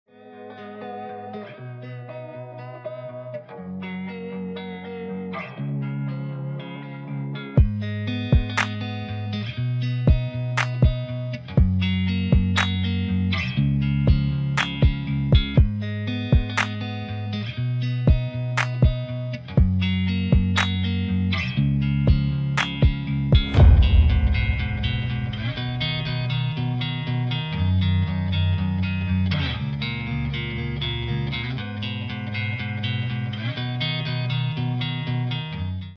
Распаковка сборной модели Танк Pz.Kpfw.III Ausf.J. (2в1) от производителя Dragon.
Масштаб - 1:35